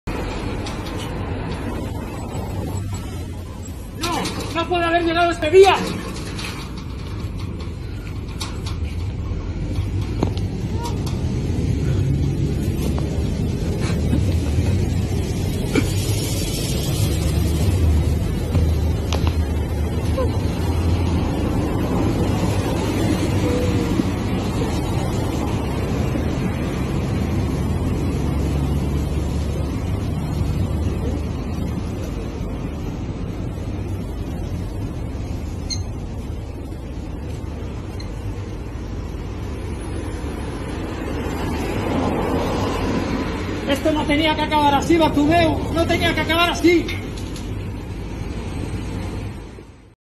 مشجع لبرشلونة ينهار بالبكاء امام بناية نادي برشلونة ويوجه رساله للادارة..
بكاء مشجع لبرشلومة امام مدخل النادي الكاتلوني حزناً على رحيل ميسي الذي قد يحصل في الايام القادمة
وهذا ما قاله للادارة.